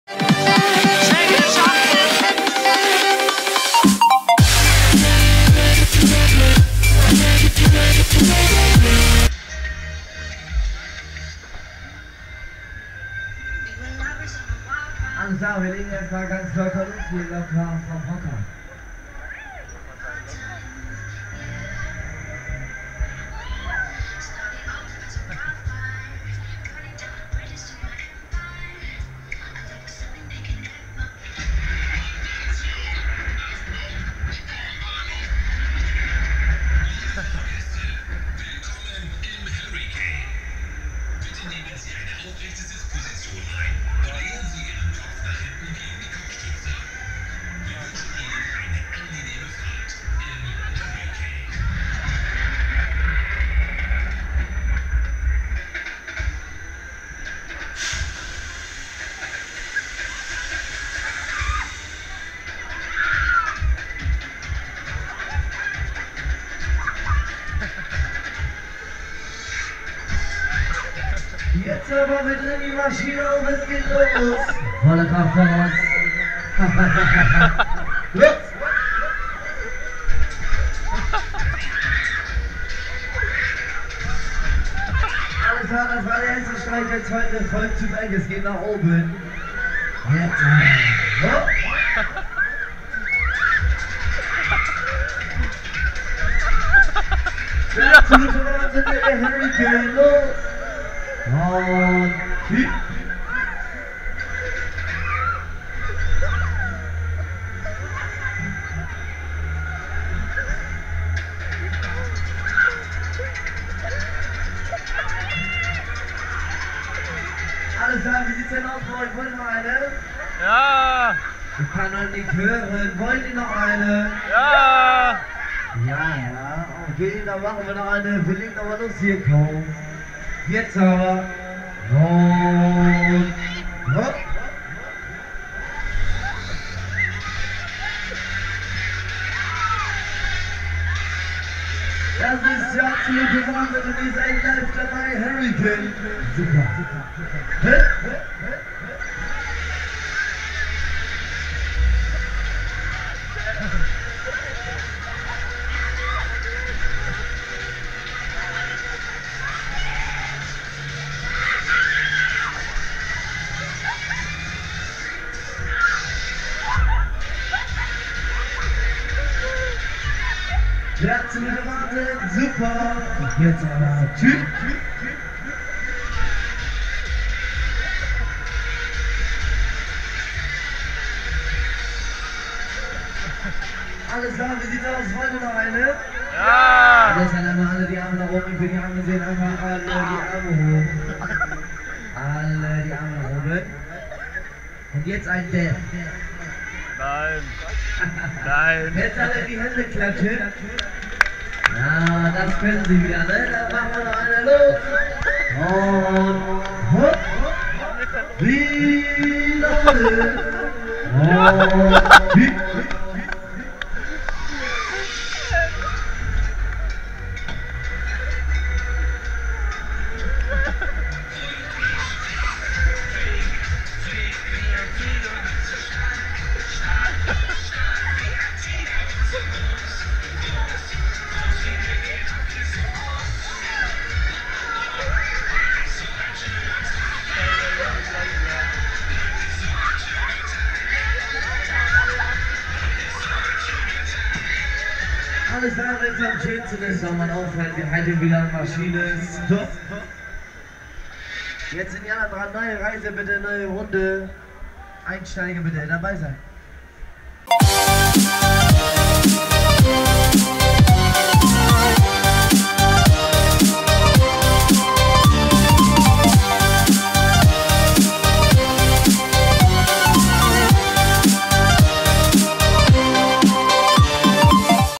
Hurricane [Heine] | Pfingstmarkt Frankenberg 2019 | OnRide POV
Hurricane auf dem Frankenberger Pfingstmarkt 2019.

Statistiken:
-Hersteller : KMG [NL]
-Typ : Fun Factory
-Inversionen : ---
-Schausteller : Karlheinz Heine [Bremen]

TheCoasterRides

Hurricane at the Pfingstmarkt in Frankenberg, Germany 2019.

Stats :
-Manufacturer : KMG [NL]
-Type : Fun Factory
-Inversions : ---
-Showman : Karlheinz Heine [Bremen, Germany]